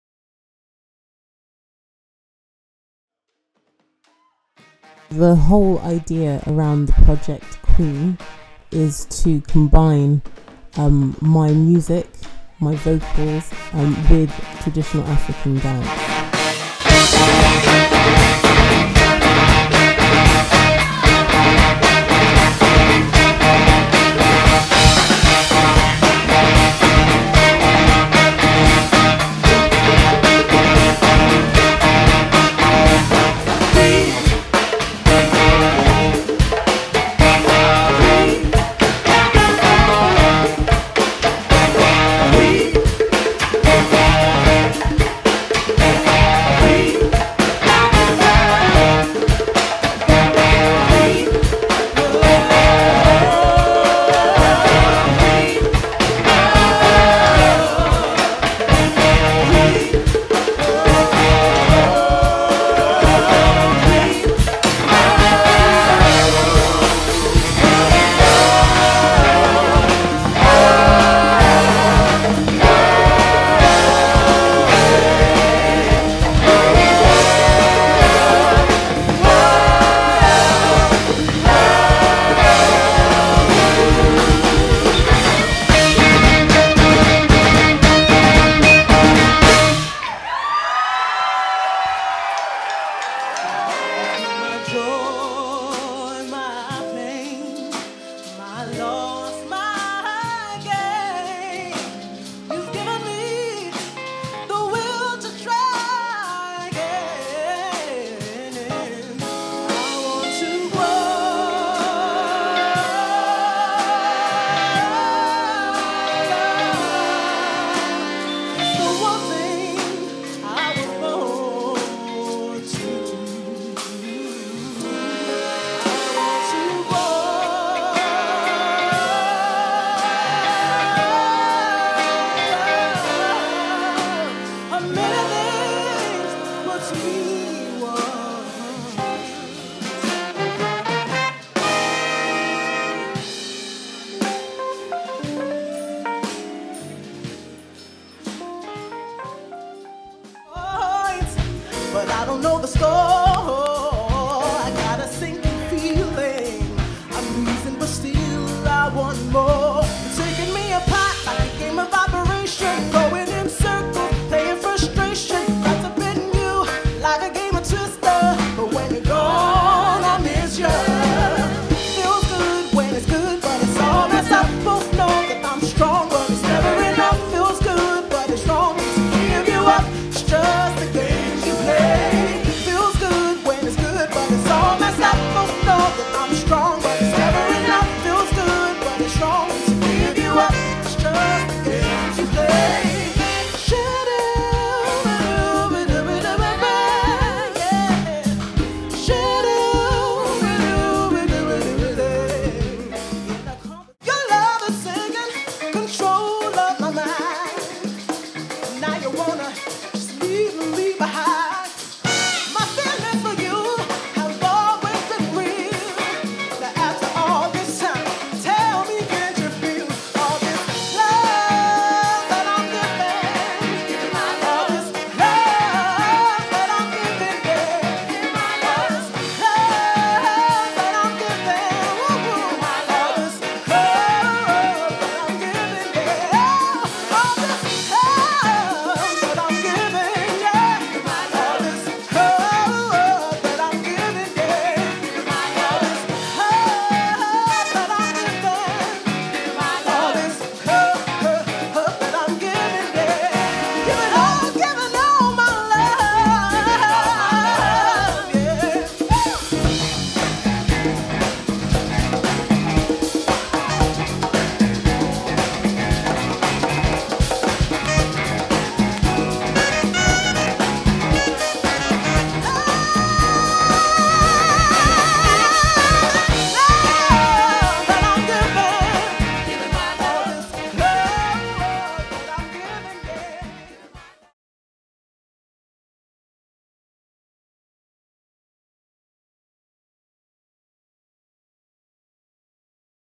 Celestine - QUEEN, a short movie
A little glimpse into who I am and what I do, excerpts taken from a short documentary showcasing the music journey and debut of my new project @ Bristol Harbourside Festival 2013. Enjoy! Xxx